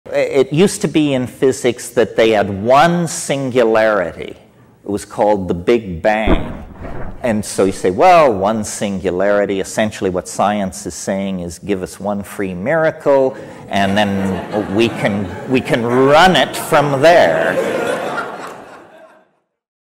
One Free Miracle - Terence McKenna
I adore Terence McKenna and of his many gems, this is one of my favorites.

“It used to be in physics that they had one singularity. It was called the big bang. And so you say, well! One singularity. Essentially what science is saying is give us one free miracle and then we can, we can run it from there.” – Terence McKenna